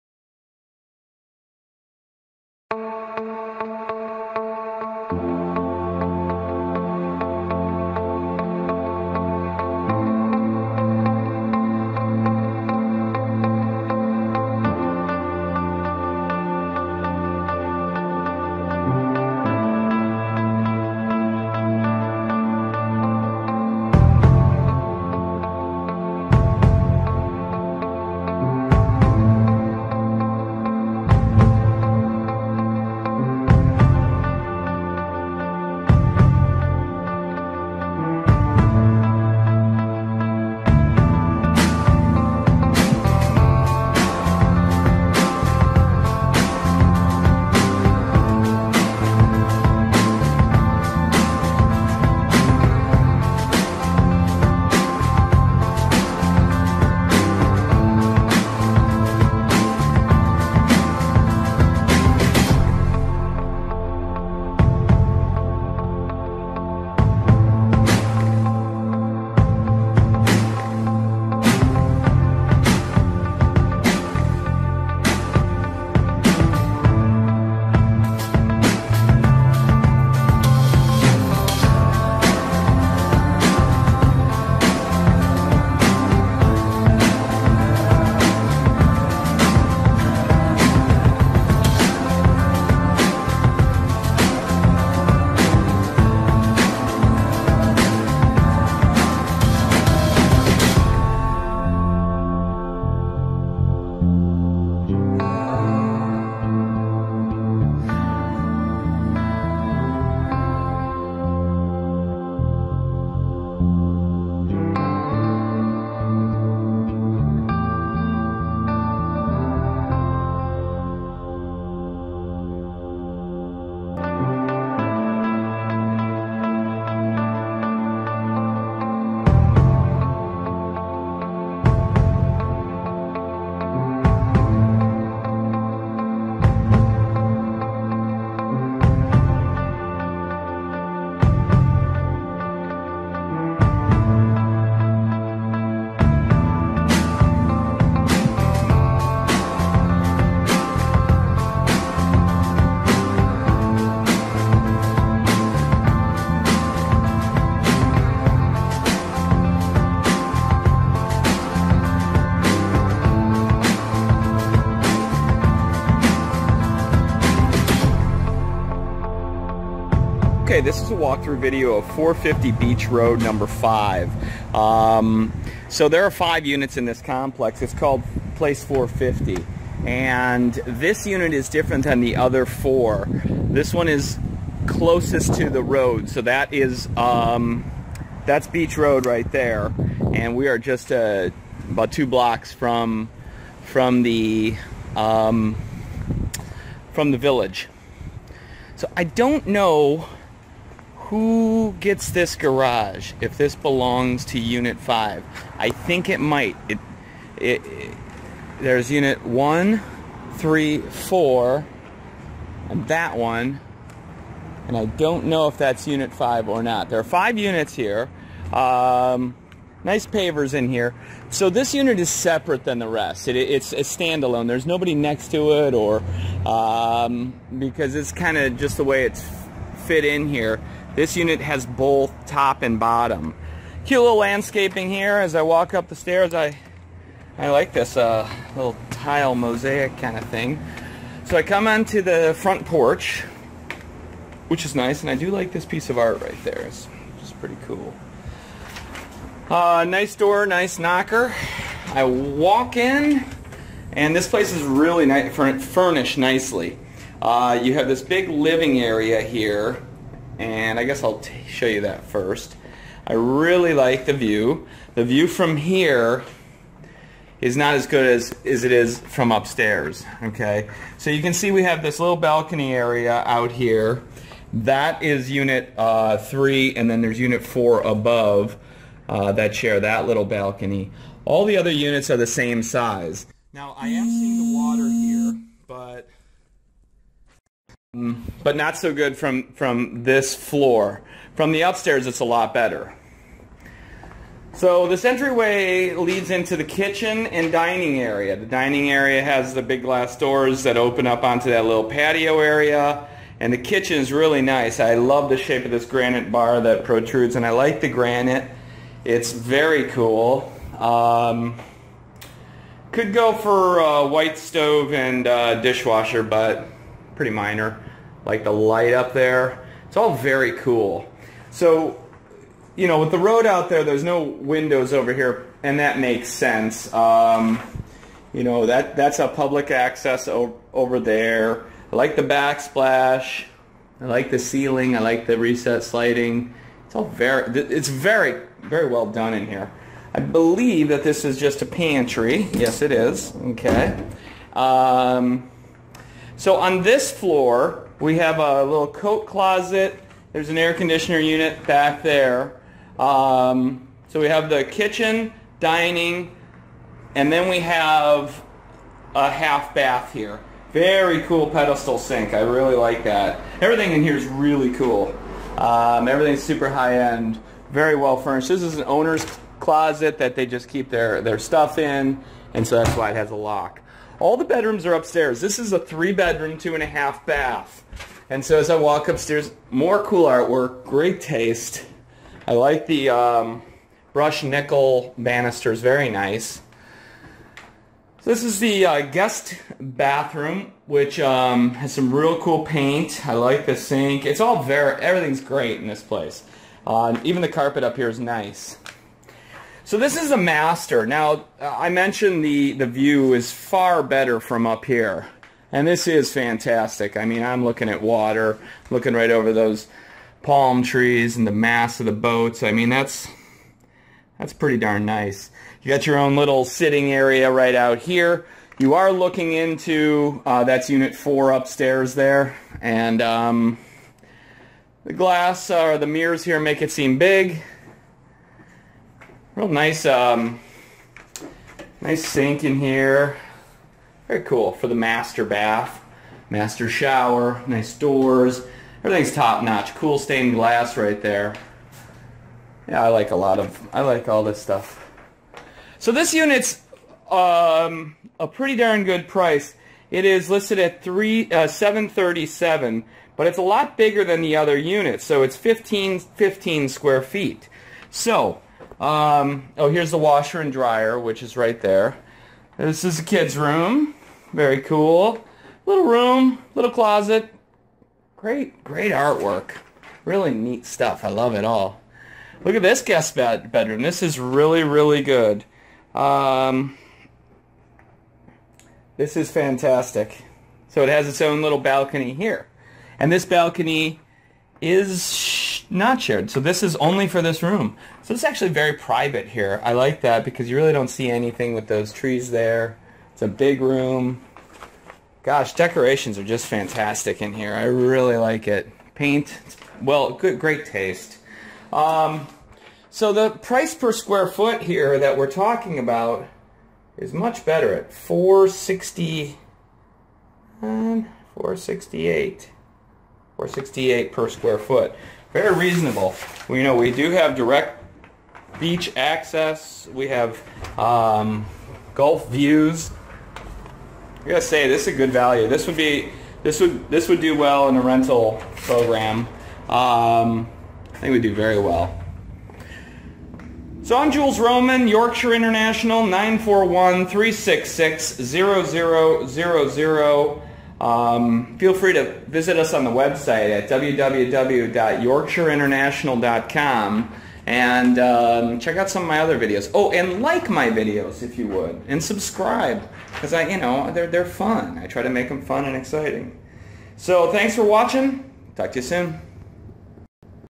450 BEACH RD, #5 Siesta Key Beachfront 737K
County: Sarasota Status: Active
Subdiv: PLACE 450
Pool: None ADOM: 112
Style: Condo CDOM: 112
Location: Balcony, Coastal Construction Control Line, Corner Unit, End Unit, In County, Street Paved
Total Acreage: Pets: Yes
Minimum Lease Period: 2 Weeks Max Times per Yr: 26
Garage/Carport: 1 Car Carport, 1 Car Garage, Assigned Parking, Attached, Covered Parking, Guest Parking, Open Parking, Parking Pad, Secured Parking, Under Building Parking

One-of-a-kind DIRECT BEACHFRONT “FREE STANDING” Home: Sink your toes in sugary white sands of America’s #1 Beach, Siesta Key Beach! This island paradise is truly unique; for those who prize exclusivity of owning a private beachfront residence, complete with endless unobstructed Gulf & Beach Views, balmy breezes & the tropical ease of Siesta Key, welcome to 450 Beach Rd. Located on highly coveted & totally desirable “Paradise Mile”, known to many, but afforded by a select privileged few, a one mile stretch of land spanning from Beach Access #11 to Beach Access #5/Ocean Boulevard. This Beach home's Island style architecture includes 4 verandahs for optimal views & welcoming the sea breezes. Upon entering the home, you are aware this is a magical location: the lush lawn & Florida fauna lead your gaze to the azure blue waters of the Gulf of Mexico. Home features: bright open floor plan, updated kitchen, granite countertops, peninsula bar, tile floors, 3 bedrooms 2.5 baths, 1 Attached Garage & 1 Attached Carport, Laundry, storage & views as far as the eye can see! Breathtaking views from your Master bedroom beachfront verandah - experience amazing sunsets & enchanting waves as they lap against the shore. Create memories with family & friends; they will always remember how much they enjoyed incredible Siesta Key & its village of shops, restaurants, bars & convenient amenities of a quaint island town known for its quartz sand beaches & friendly island lifestyle. 2 week min. rental AND Pet friendly!